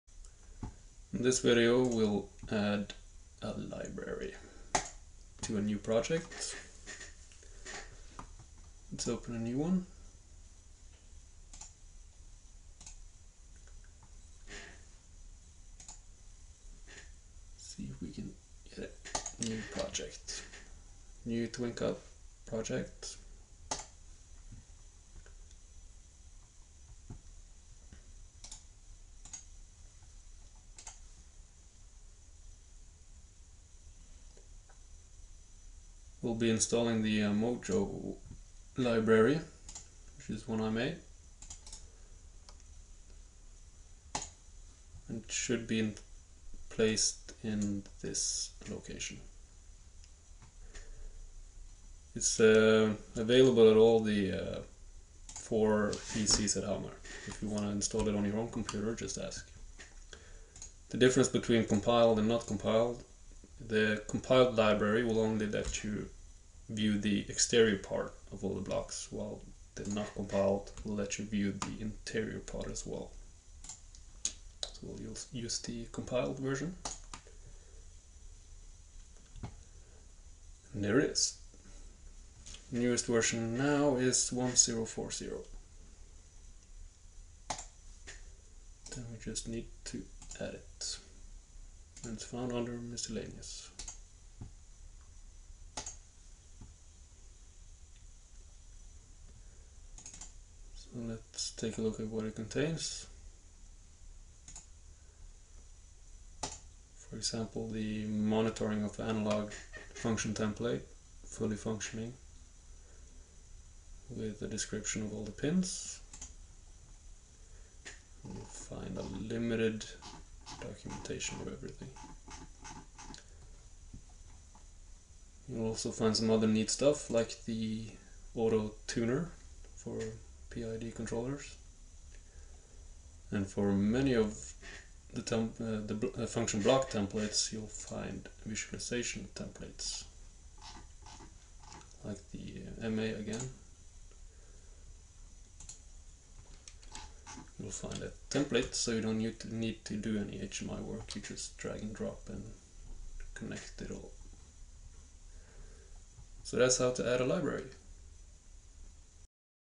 TC 01 New Project Add Library
Instruction video for the course EL2-202 Process Control.
How to create a new project in TwinCAT and add a library.